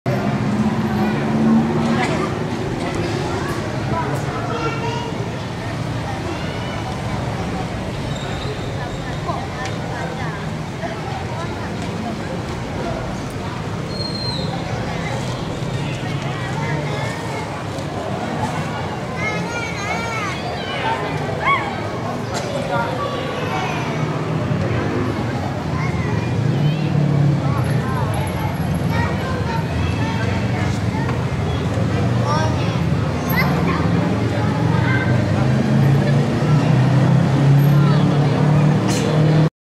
suasana ramai di mesjid jamik malang sabtu 14 januari 2017
usai salat magrib